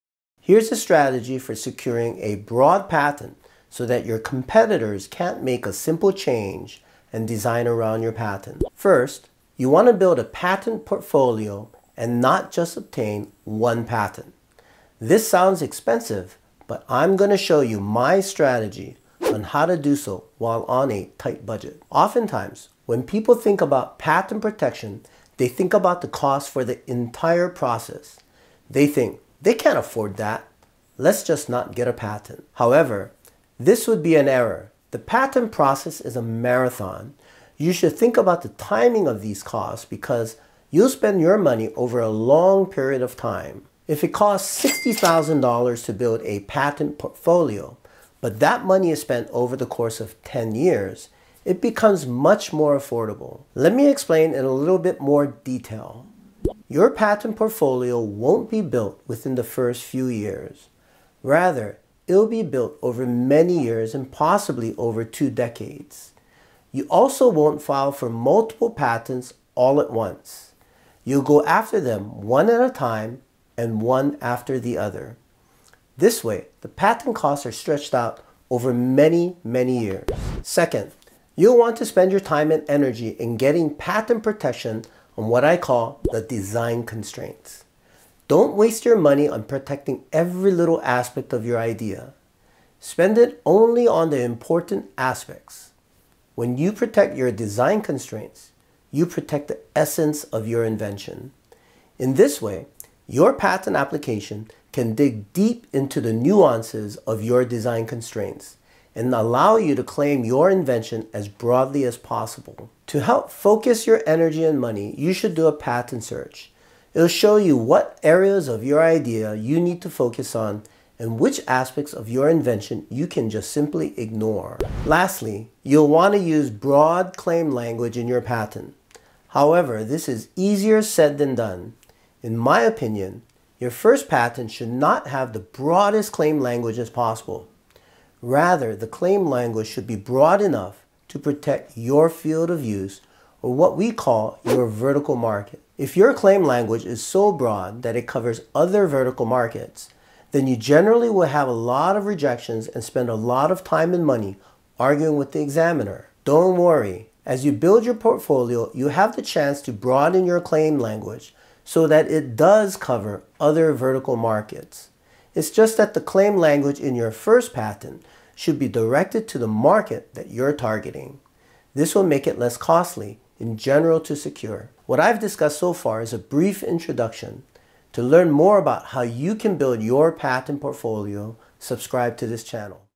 How to secure broad patent protection
To secure a broad patent, you should:

1. Build a portfolio;
2. Protect the design constraints; and
3. Use broad claim language to cover your vertical market.